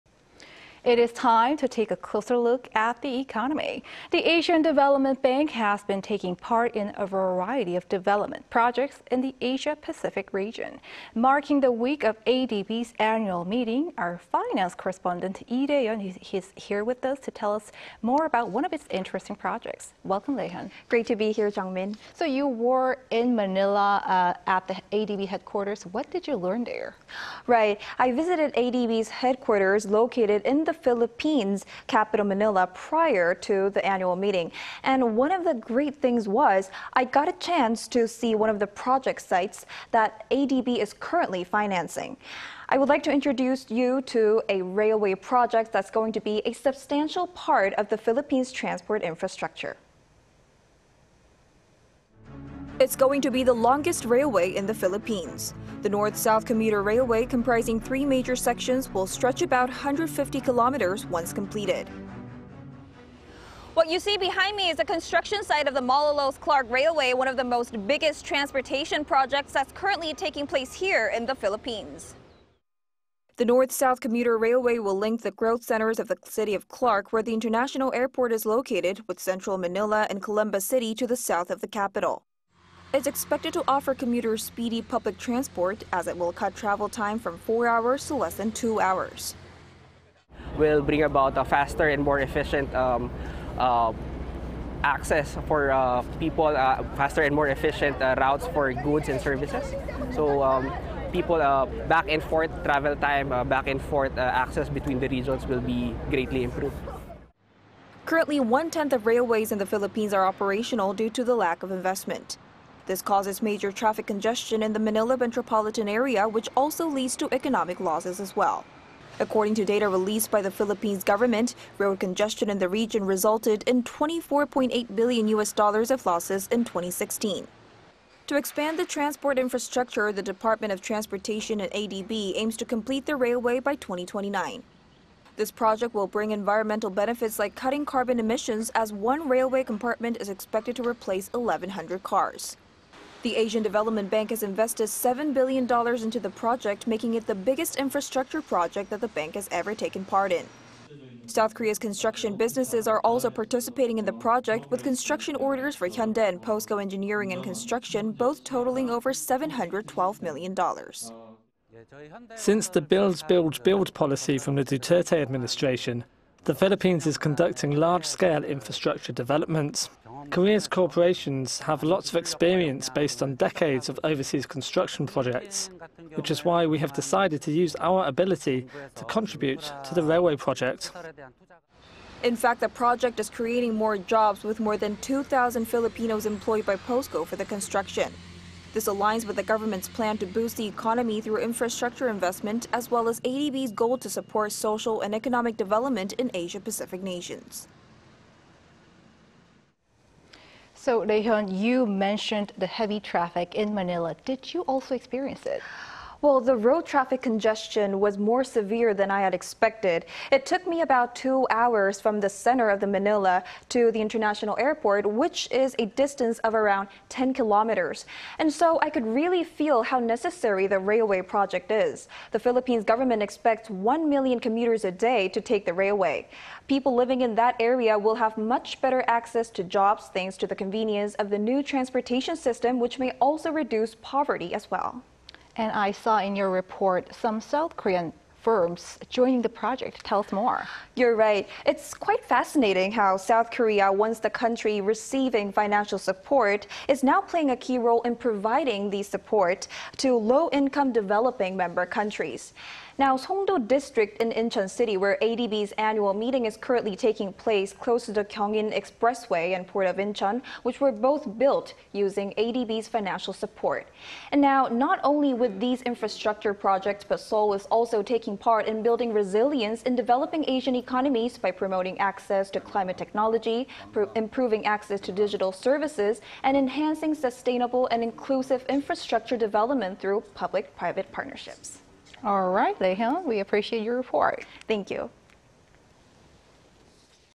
ADB investment helps fund Philippines North-South Commuter Railway
"필리핀에서 가장 긴 철도" ADB 인프라 프로젝트 남북철도

It's time to take a closer look at the economy.
The Asian Development Bank has been taking part in a variety of development projects in the Asia-Pacific region.
Marking the week of ADB's annual meeting our finance correspondent Lee Rae-hyun is here with us to tell us more about its projects.
Welcome, Rae-hyun.

Great to be here, Jung-min.

So, Rae-hyun, you visited the ADB headquarters in Manila last month, tell us about your time there?

Right, I visited ADB's headquarters located in the Philippines capital Manila prior to the annual meeting.
And one of the great things was, I got a chance to see one of the project sites that ADB is currently financing.
I would like to introduce you to a railway project that's going to be a substantial part of the Philippines' transport infrastructure.

It's going to be the longest railway in the Philippines.
The North-South Commuter Railway comprising three major sections will stretch about 150 kilometers, once completed.

"What you see behind me is the construction site of the Malolos Clark railway, one of the biggest transportation projects that is currently taking place in the Philippines."

The North-South Commuter Railway will link the growth centers of the city of Clark, where the international airport is located with central Manila and Calamba city to the south of the capital.
It's expected to offer commuters speedy public transport as it will cut travel time from 4 hours to less than 2 hours.

"We'll bring about faster and more efficient access for people, faster and more efficient routes for goods and services, to people back and forth travel time back and forth access between the regions will be pretty clear."

Currently, one-tenth of railways in the Philippines are operational due to the lack of investment.
This causes major traffic congestion in the Manila metropolitan area which also leads to economic losses as well.
According to data released by the Philippines government, road congestion in the region resulted in 24-point-8 billion U.S. dollars of losses in 2016.
To expand the transport infrastructure, the Department of Transportation and ADB aims to complete the railway by 2029.
This project will bring environmental benefits like cutting carbon emissions as one railway compartment is expected to replace eleven hundred cars.
The Asian Development Bank has invested 7 billion dollars into the project making it the biggest infrastructure project that the bank has ever taken part in.
South Korea's construction businesses are also participating in the project with construction orders for Hyundai and POSCO Engineering and Construction both totaling over 712 million dollars.

"Since the build, build, build policy from the Duterte administration, the Philippines is conducting large-scale infrastructure development. Korea's corporates have lots of experience based on decades of overseas construction projects which is why we have decided to use our ability to contribute to the railway project."

In fact, the project is creating more jobs with more than 2000 Filipinos employed by POSCO for the construction.
This aligns with the government's plan to boost the economy through infrastructure investment as well as ADB's goal to support social and economic development in Asia-Pacific nations.

Right, so you've mentioned the heavy traffic in Manila and you actually experienced it during the stay.
Can you describe that?

Well, the road traffic congestion was more severe than I had expected.
It took me about 2 hours from the center of Manila to the international airport a distance of around 10 kilometers.
And so, I could really feel how necessary the railway project is.
The Philippines government expects 1 million commuters a day to take the railway.
People living in that area will have much better access to jobs thanks to the convenience of the new transportation system which may also reduce poverty as well.

And as we heard, some South Korean companies are also taking part in the project. Give us some details...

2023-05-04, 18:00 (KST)